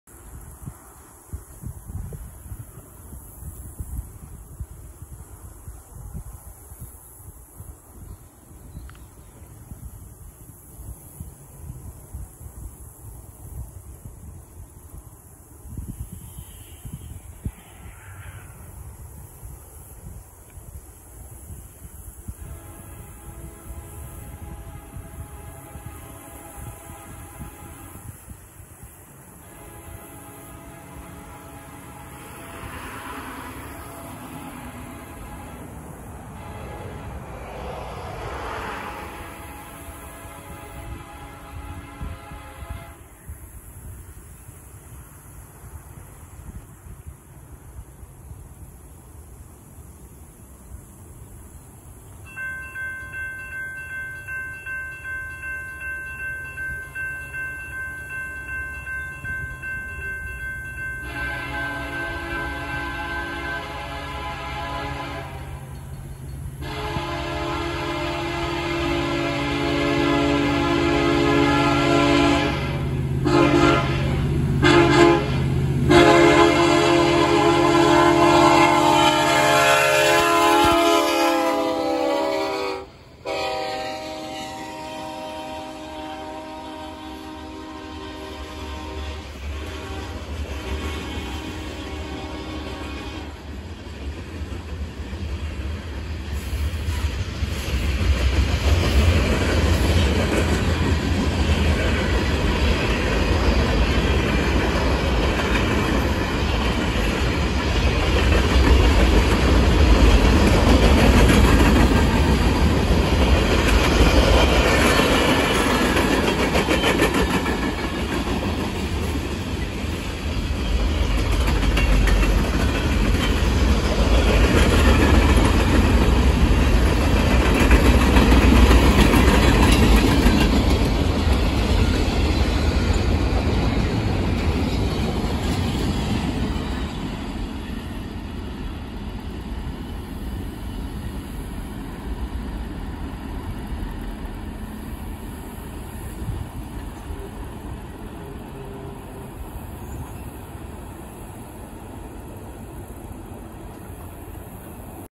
K5LLA Hornshow! KCS train MDASH at Copeville, TX (September 17, 2021)
KCS 4052 heads east with the MDASH on the Greenville Sub at Copeville. KCS 4049 and 1909 were trailing units. The MDASH is a manifest running from Dallas, TX to Shreveport, LA. This train also had a friendly crew, as they put on a horn show for me as they approached the crossing.

William Biedenbender Videos 2021 (Duncan_Rail_Videos)